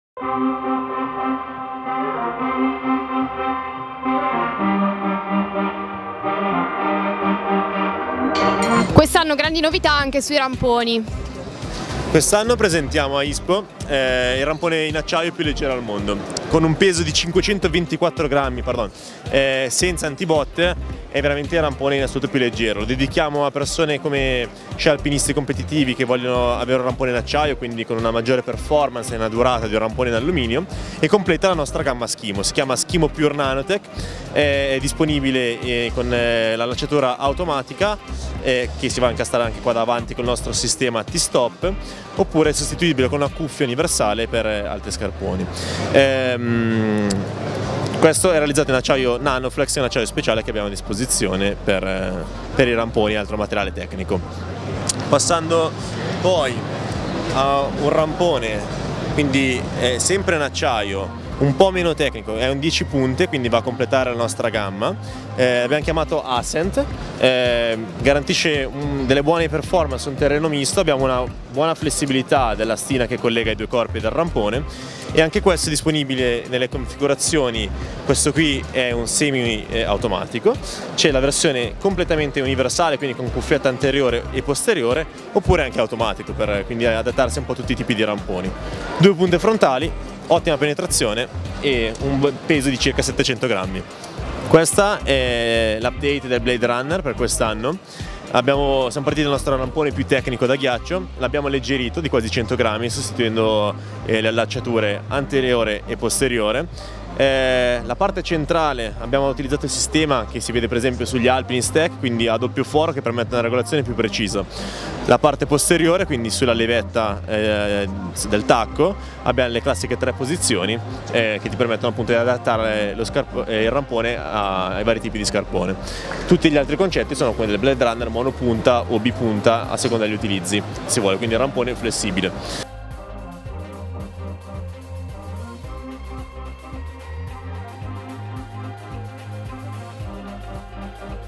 CAMP Skimo Pure Nanotech, Ascent, nuovo Blade Runner - Oliunìd @ ISPO 2020
Allo stand di CAMP le novità non sono più finite: Skimo Pure Nanotech è il rampone in acciaio più leggero al mondo, pensato dunque per lo scialpinismo.
Ascent è invece un rampone a 10 punte, sempre in acciaio, ottimo su terreno misto grazie alla buona flessibilità dell'astina.
Blade Runner rinnovato!